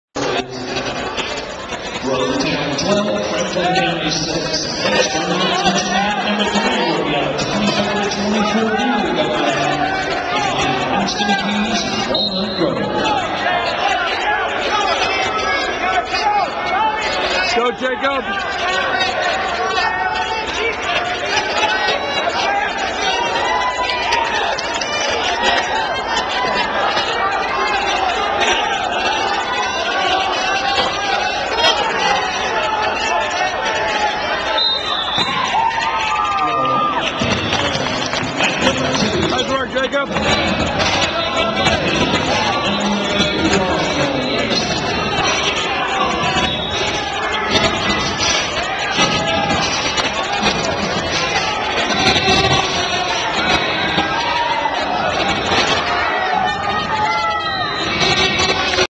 Lambert High School - New Schools Duel, match 1
Jacob wins first match of tournament with a pin.